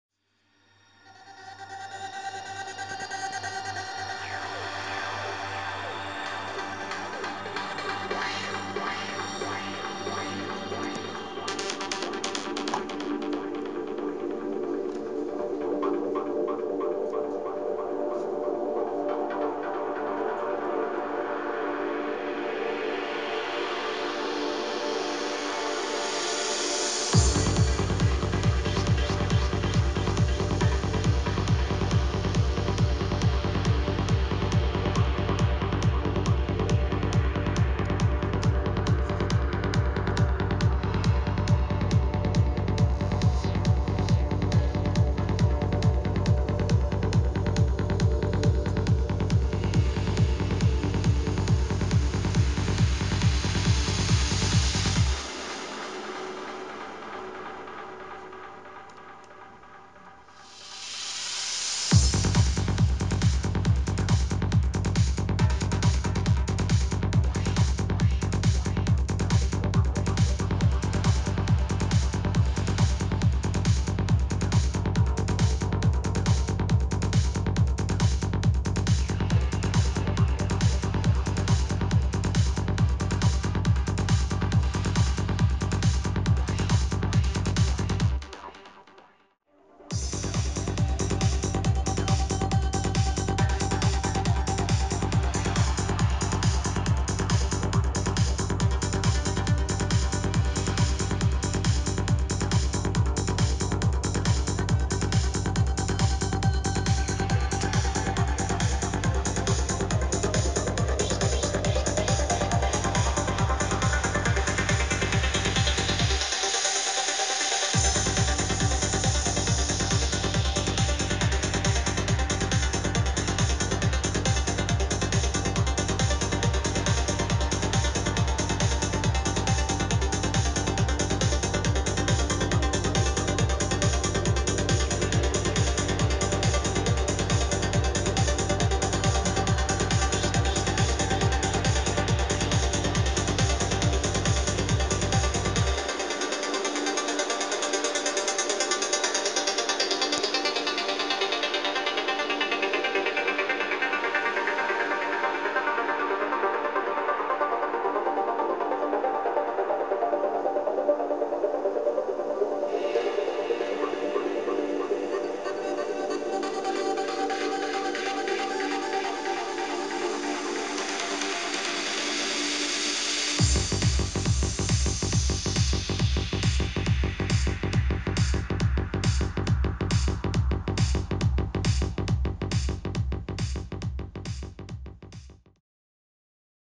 Norma Project & Liquid Sound new track in progress (sneak peak)
New track in progress with my friend Norma Project.

Enjoy in this sneak peak :)